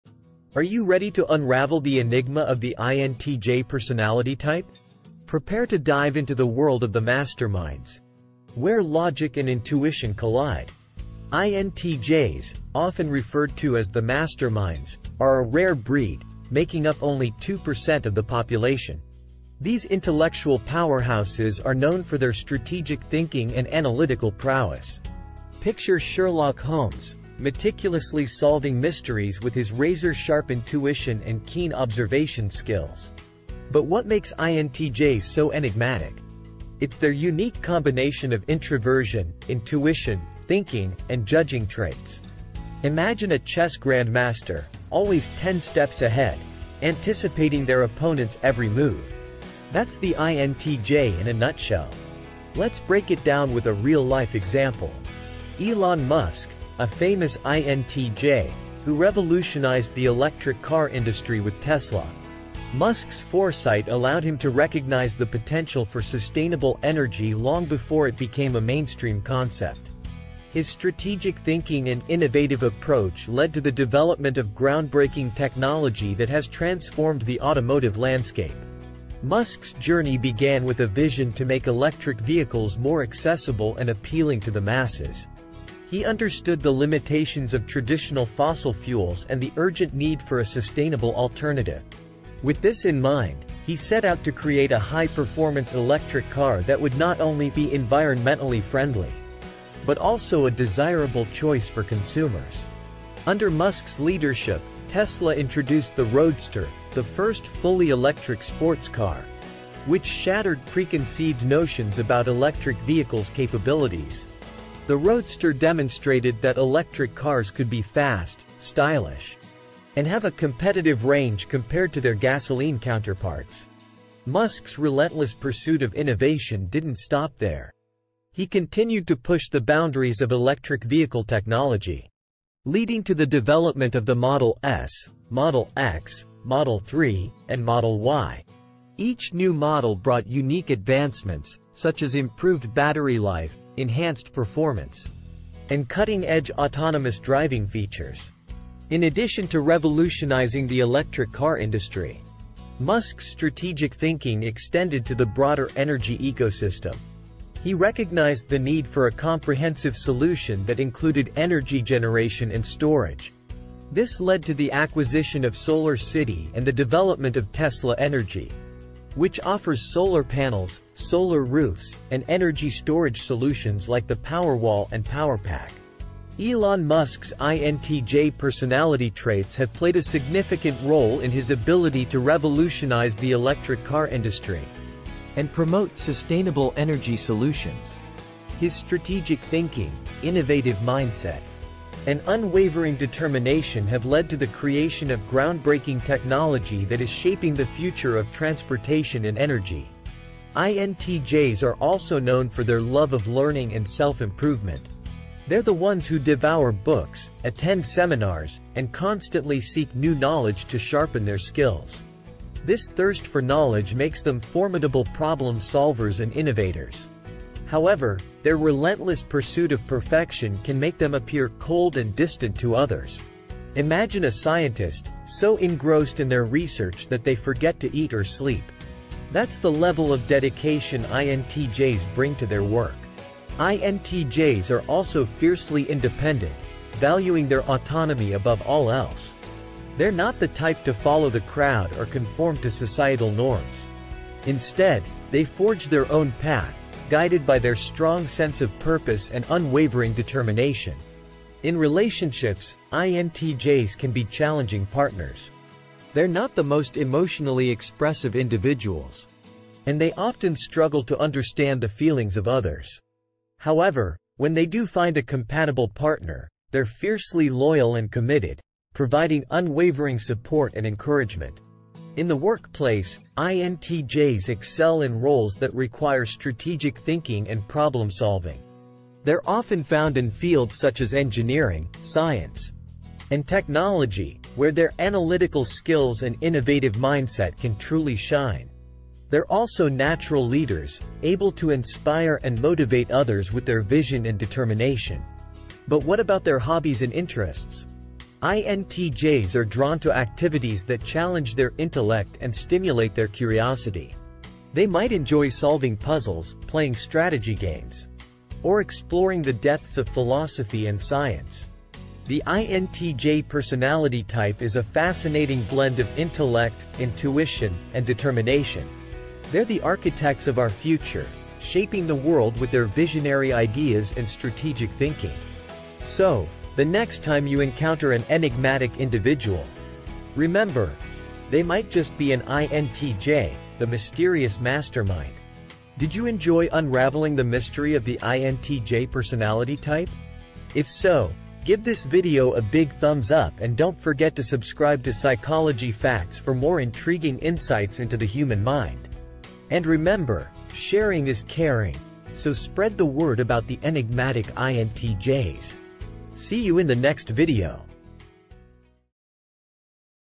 INTJ: The mysterious personality type
In this video, we're going to explore the INTJ personality type – one of the most mysterious and misunderstood personality types out there.

INTJs are unique individuals with a lot of personal development to ...